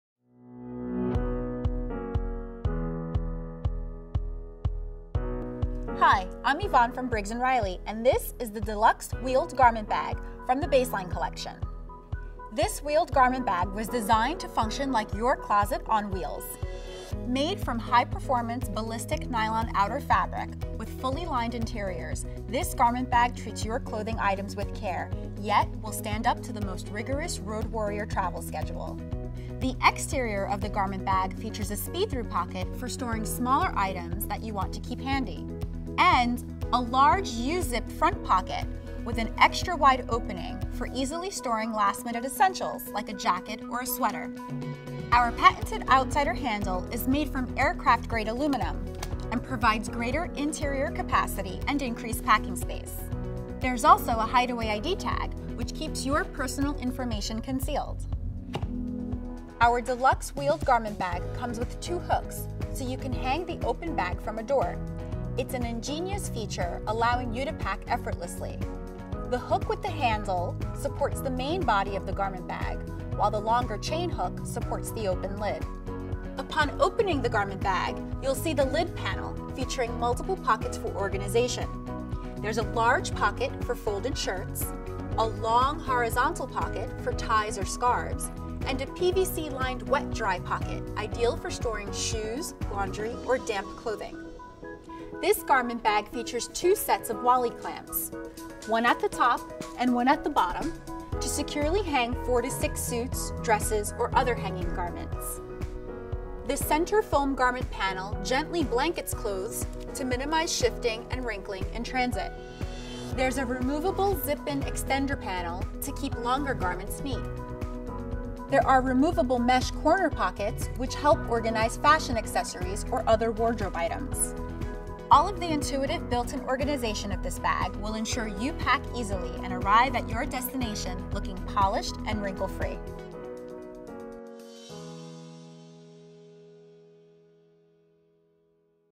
A Versatile Garment Bag for Professional or Personal Travel
This garment bag has a fully lined interior with a zip around door panel, two sets of Wally clamps to hang 4 to 6 suits, two mesh pockets, a zippered shoe pocket, a scarf pocket, corner pockets, and a zip in extender panel to keep longer garments neat.